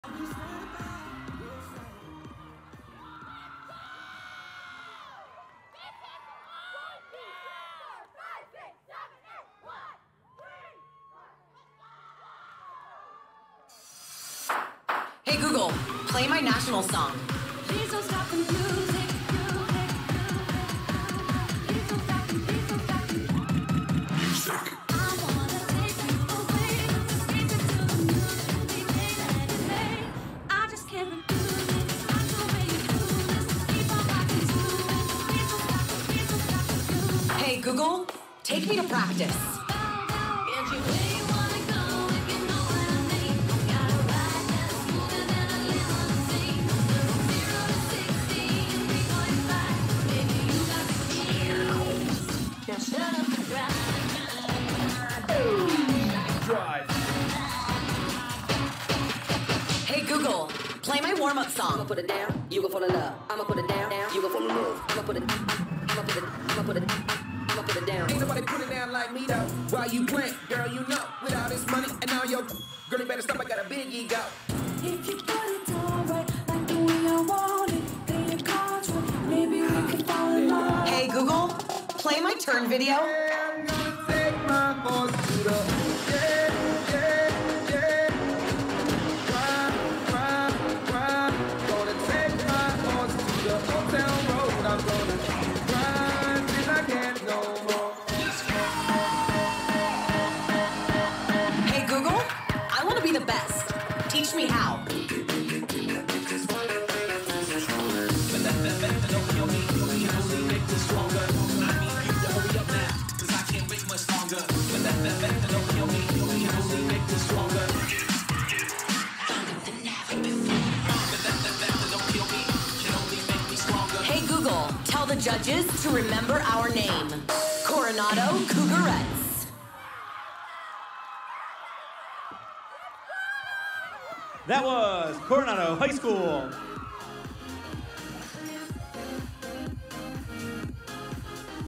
Coronado Cougarettes 1st Place Hey Google Pom
National Campions NDA Nationals
Choreographed by Bre and Jackie